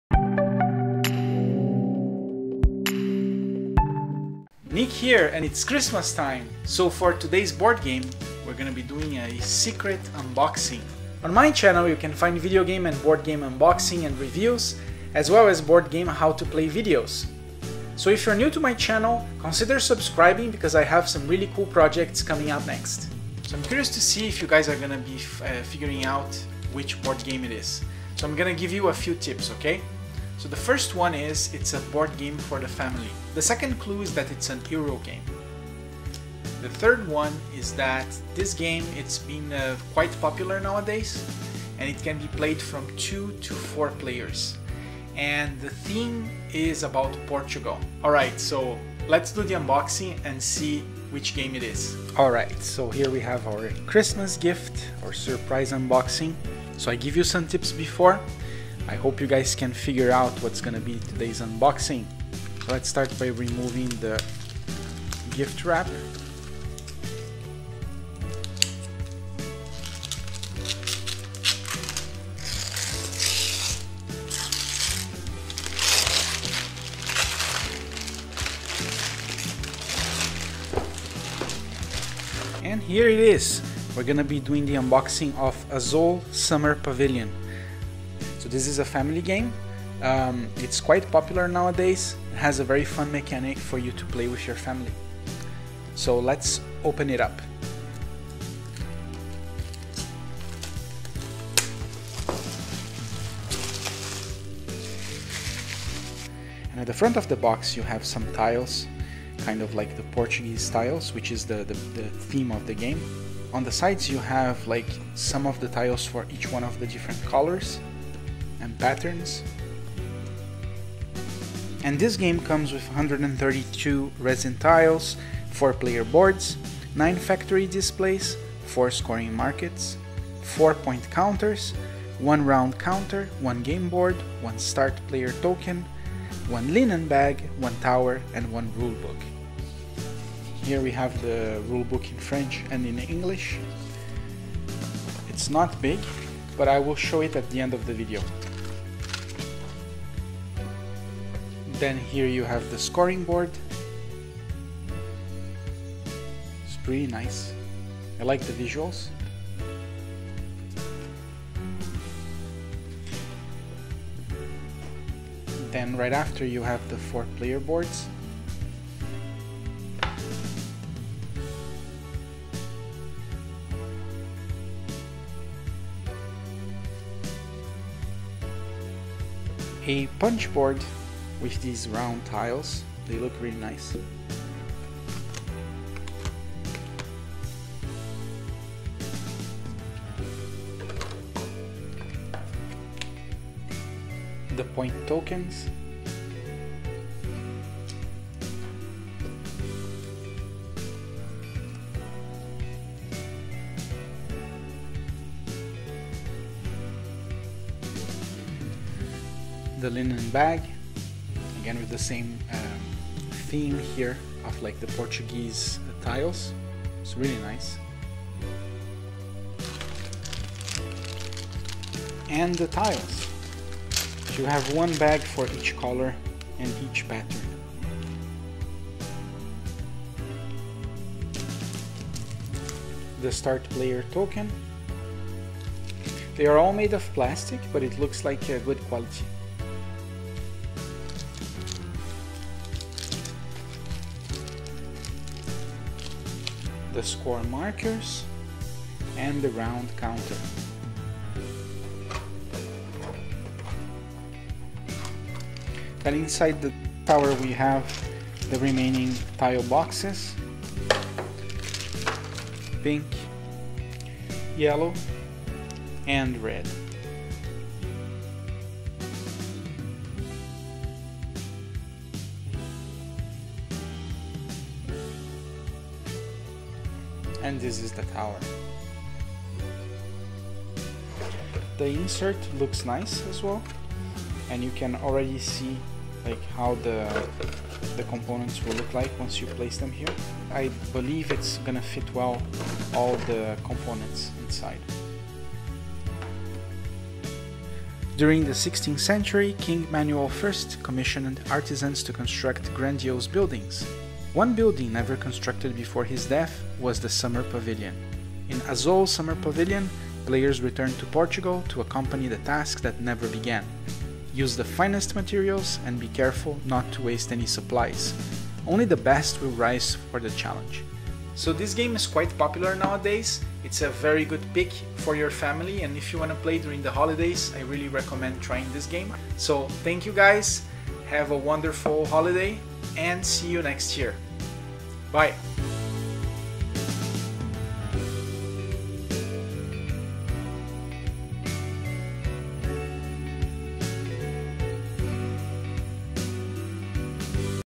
Surprise Christmas Board Game Unboxing!
Nick here!
Today we have a surprise Christmas unboxing for you. I picked a family game since we like to spend some time with our families during the holidays and this one is on the list of best board games in the last 2 years.

I highly recommend this board game to anyone :)

Cheers